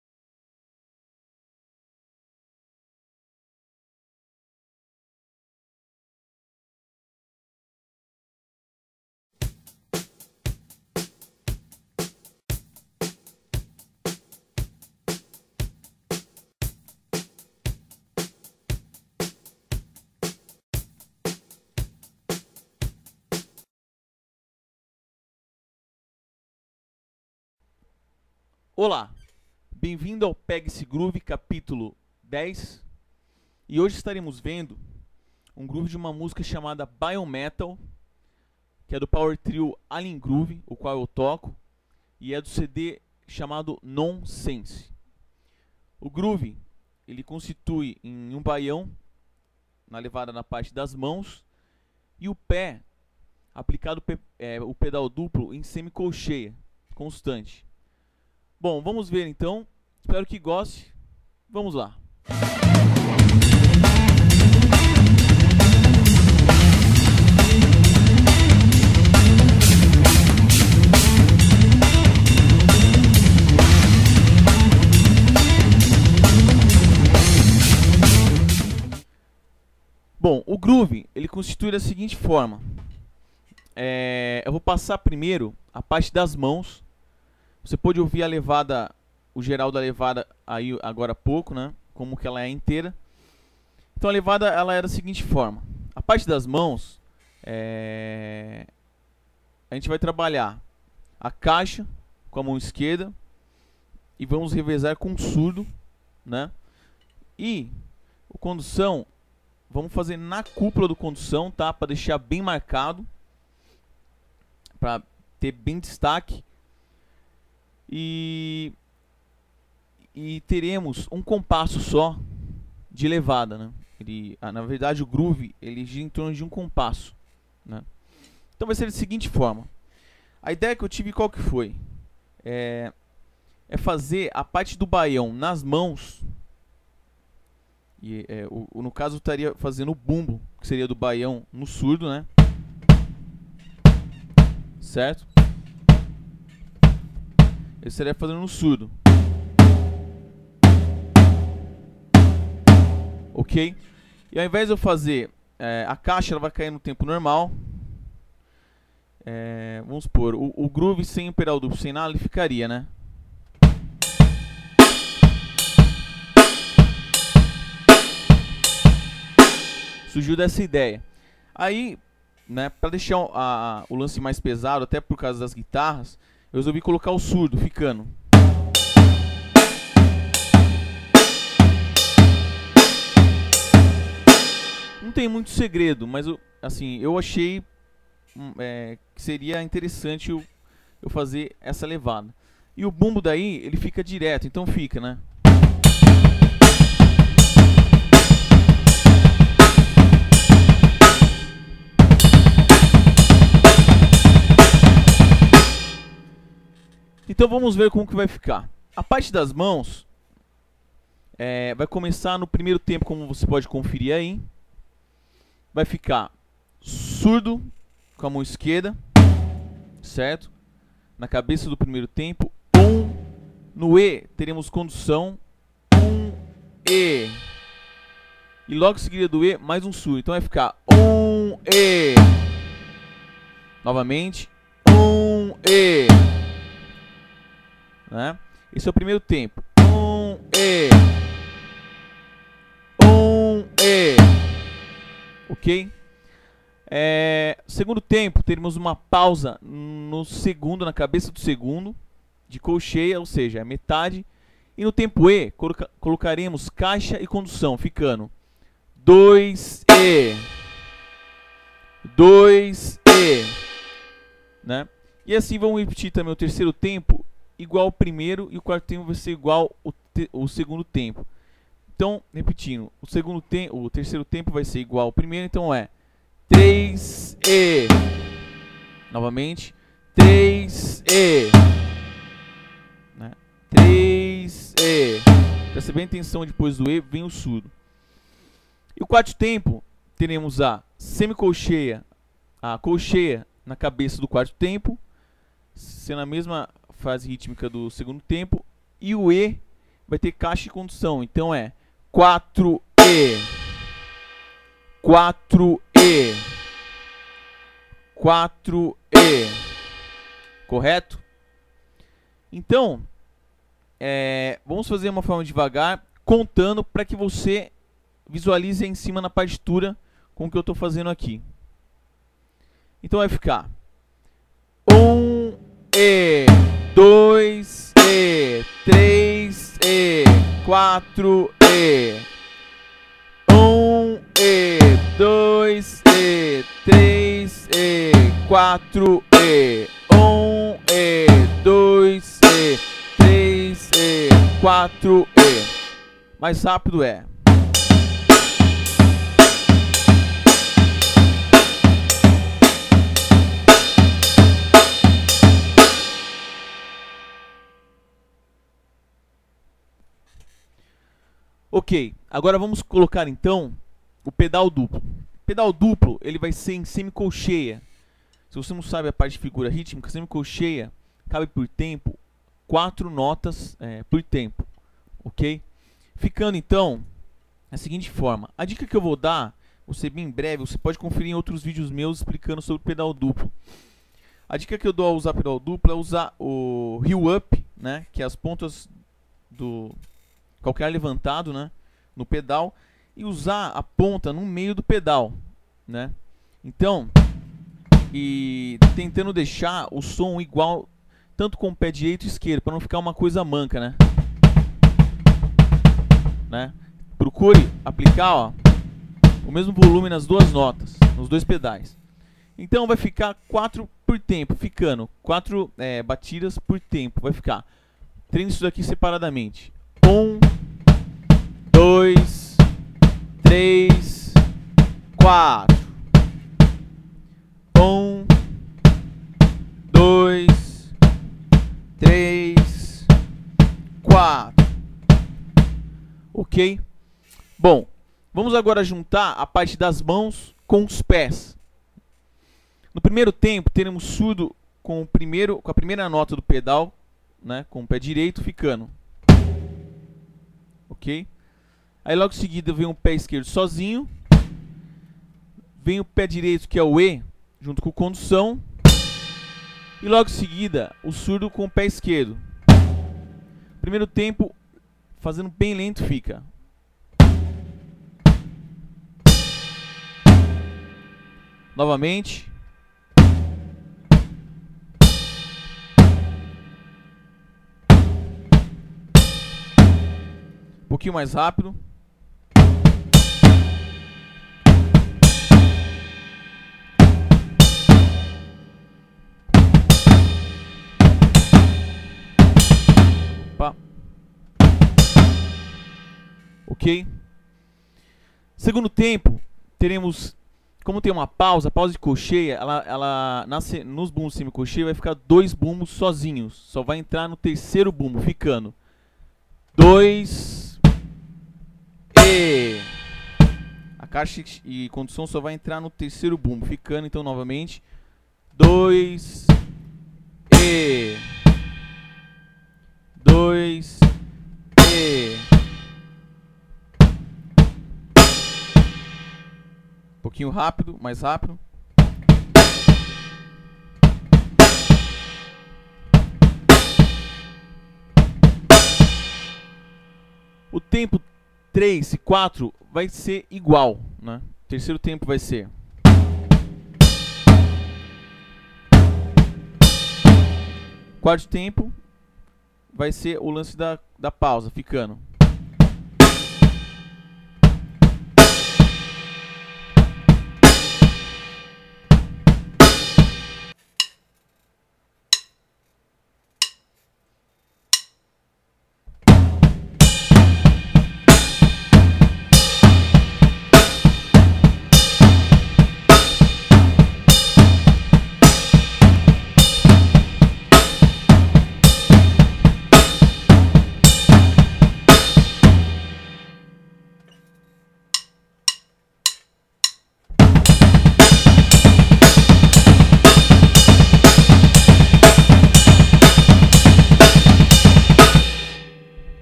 Gerson Lima Filho - Pegue esse Groove!!! (Baião Metal) - 10
" Pegue esse Groove" desenvolvida pelo Gerson Lima Filho o qual, cada video ensina um ou dois ritmos para que você possa ampliar seu vocabulário musical!!!